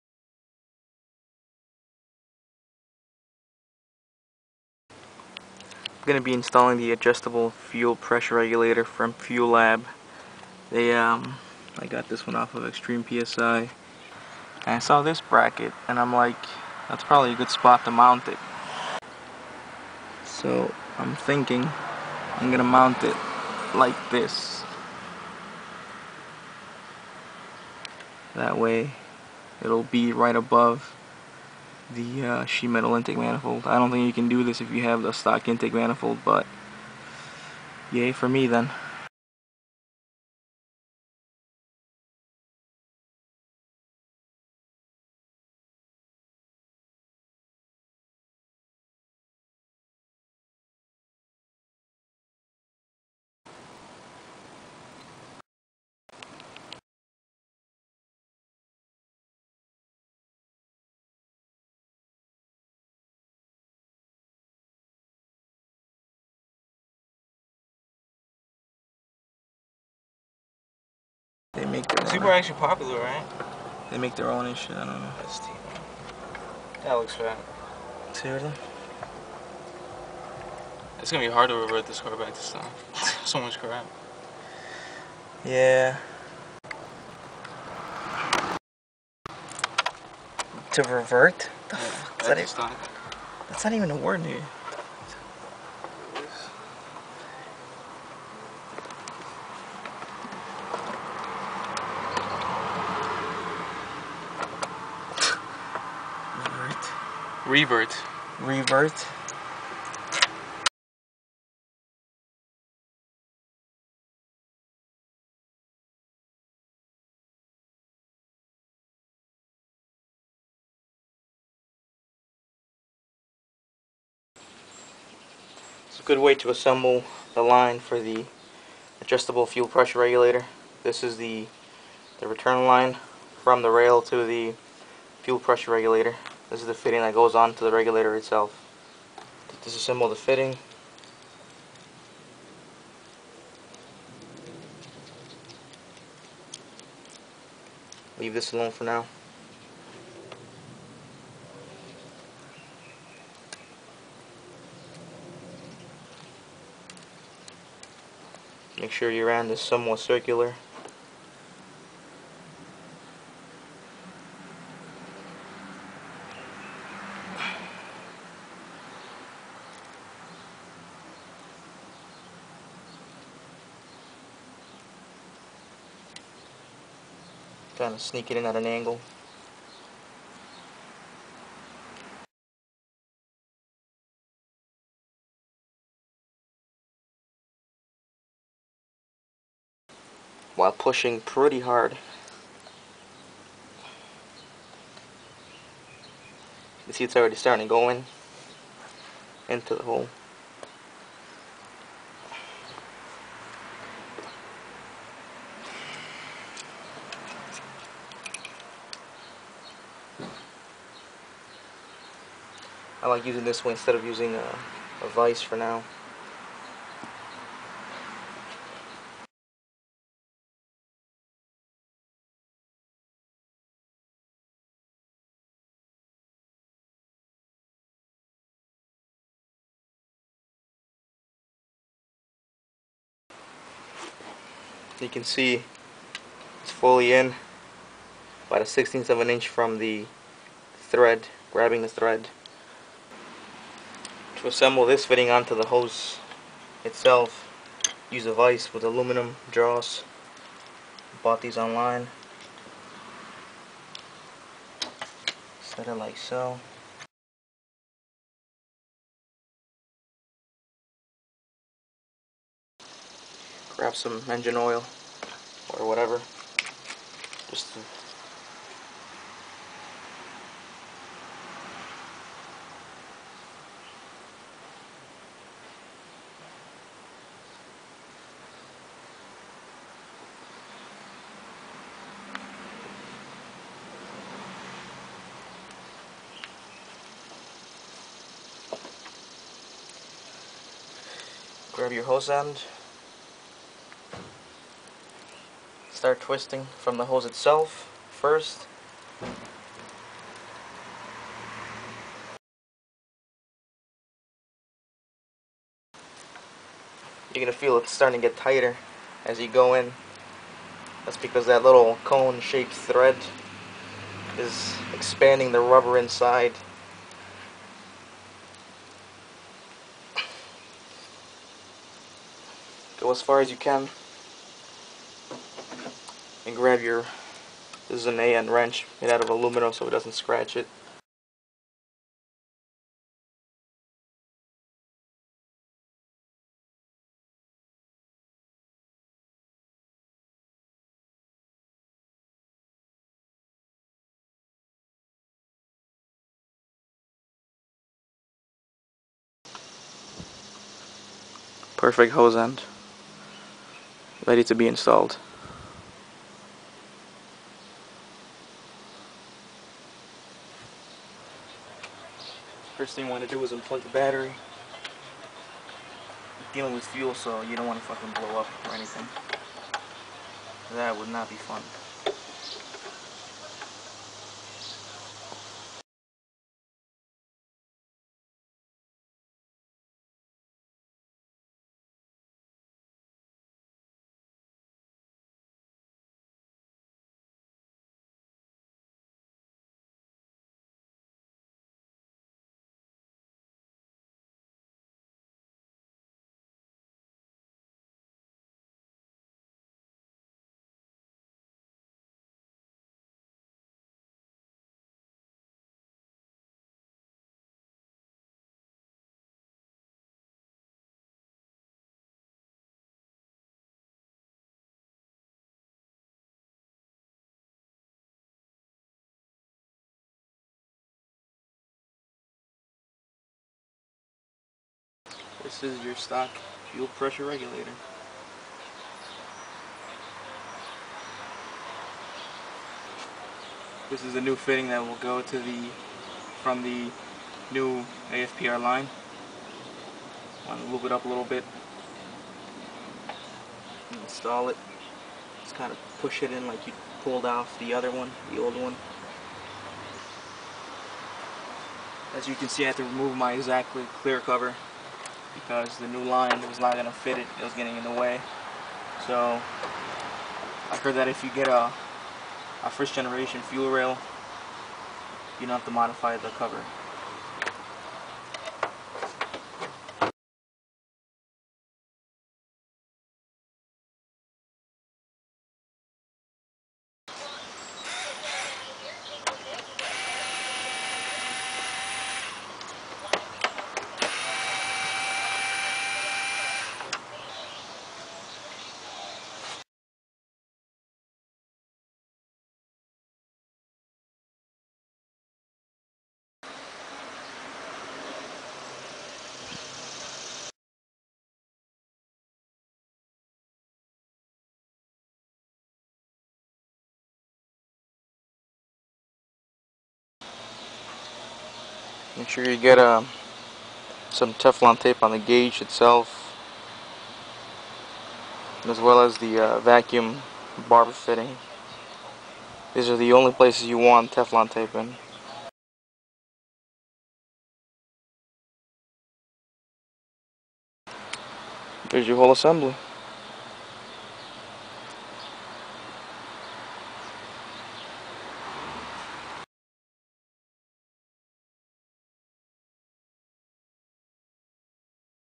Adjustable Fuel Pressure Regulator aka AFPR install - Part 1/2
This vid took me way too long to make. Too much little editing crap. lol

But as the title states, I'm installing it on my 1999 Mitsubishi Eclipse GSX so I can finally almost complete the fuel mods I can start with the DSMLink/ECMLink V3 tuning. This how to for the installation is very straight forward.

The video also depicts good techniques on how to assemble stainless steel braided hose/line for all you -AN fittings which you can appy to push lock applications as well.

The actual adjustment of the AFPR can be found onine on DSMTuners or something but since I haven't compiled Part two, I may include that section.

Since the kit is is almost universal it can be applied to many cars like Honda Toyota Subaru Ford Chevy etc.

Good luck and happy boosting